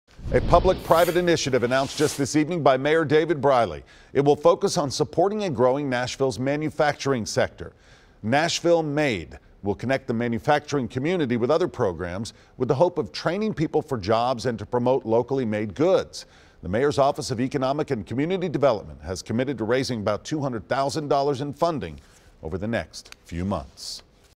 NashvilleMade initiative
A new initiative that plans to help the local manufacturing industry was unveiled by Mayor Briley this evening.